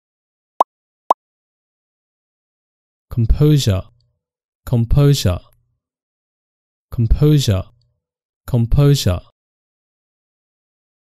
How to Pronounce Composure
In this video we'll show you how to correctly pronounce:  Composure